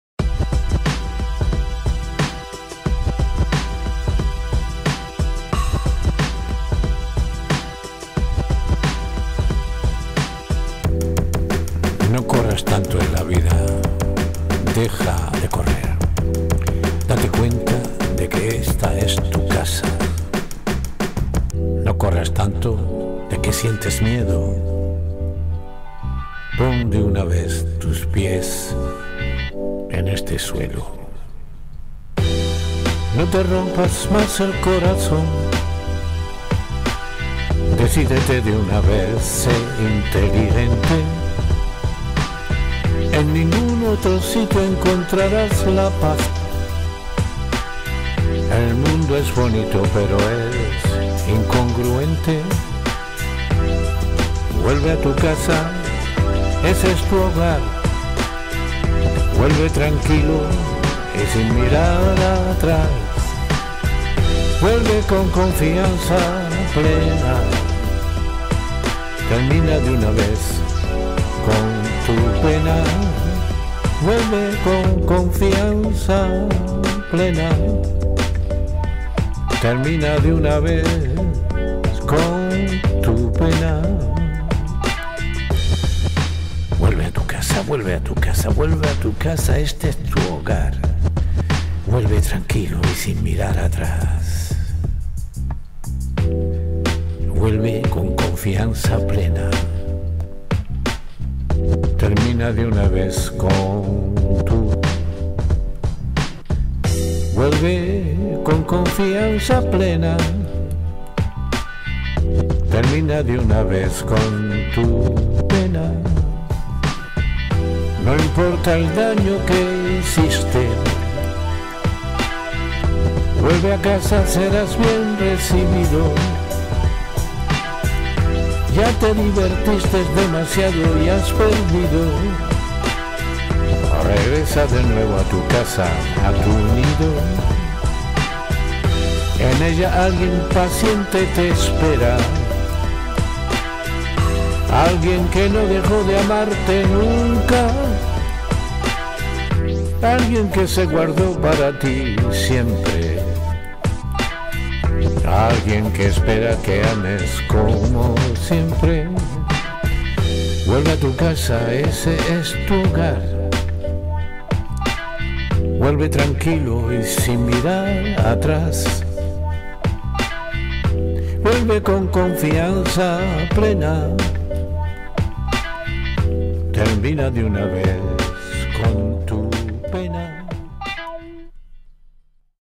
Deja de correr
No corras tanto en la vida deja de correr
Date cuenta de que esta es tu casa
No corras tanto de que sientes miedo
Pon de una vez tus pies en este suelo

No te rompas mas el corazon
Decidete de una vez, se inteligente
En ningun otro sitio encontraras  la paz
El mundo es bonito pero es incongruente

Vuelve a tu casa, ese es tu hogar
Vuelve tranquilo y si mirar atras
Vuelve con confianza plena
Termina de una vez con tu pena

No importa el daño que hiciste
Vuelve a casa seras bien recibido
Ya te dibertiste demasiado y has perdido
Regresa de nuevo a tu casa, a tu nido

En ella alguien paciente te espera
Alguien que no dejo de amarte nunca
Alguien que se guardo para ti siempre
Alguien que espera que ames como siempre

Vuelve a tu casa, esse es tu hogar